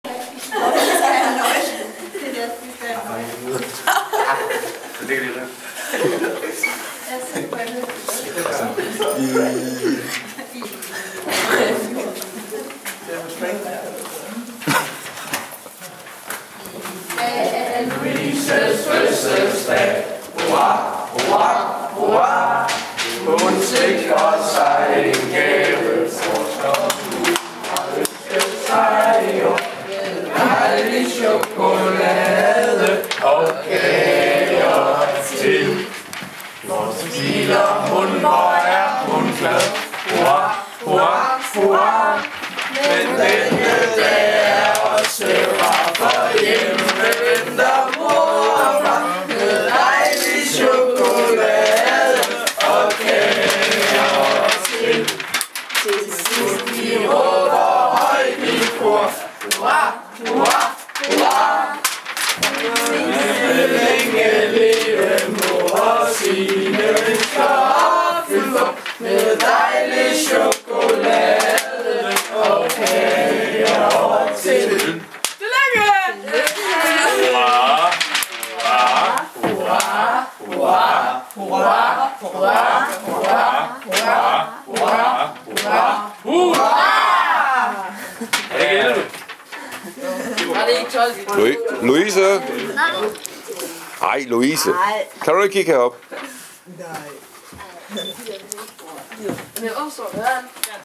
Louise fylder år på træningslejr i Løkken
Louises 11 års fødselsdag på træningslejr i Løkken d. 6/5-2011